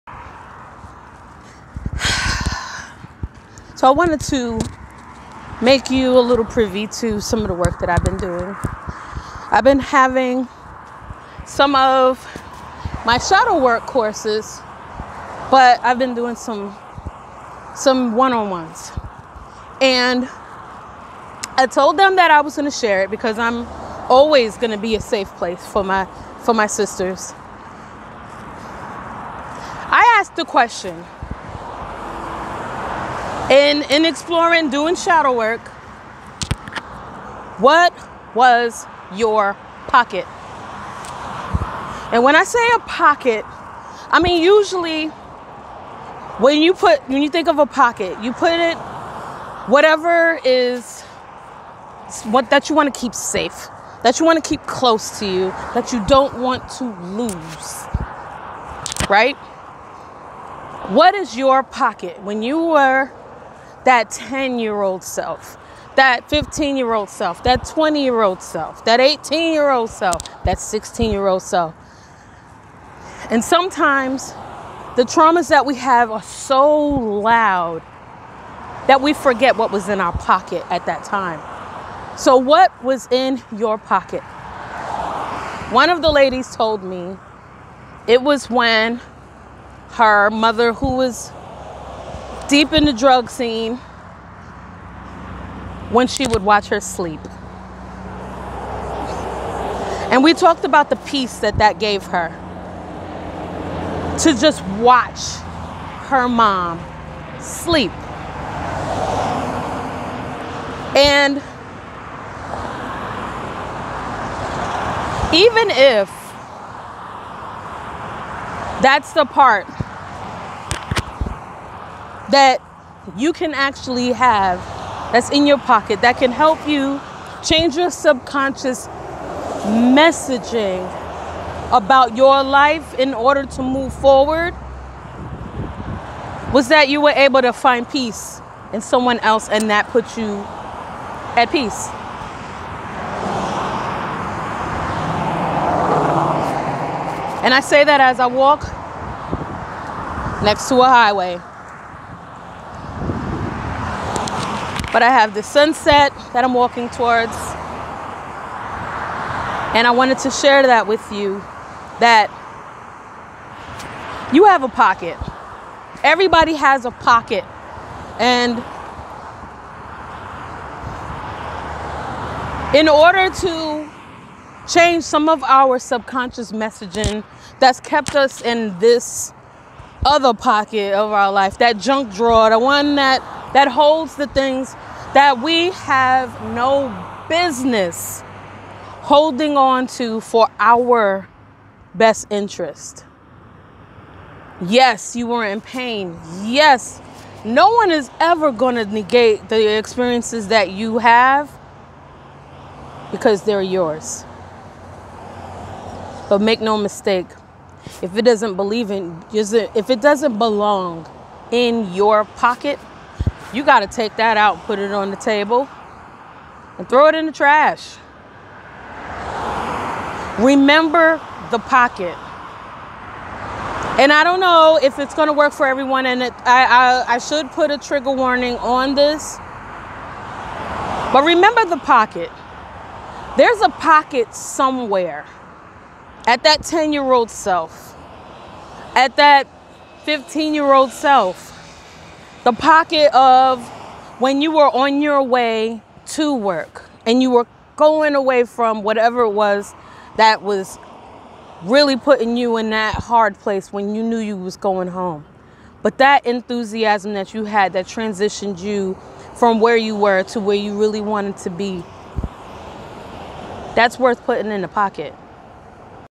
Face Your Shadow - Get it out of the pocket
Book, Lifestyle and Respire Coach, Birdie Chesson gives her motivational pep-talk.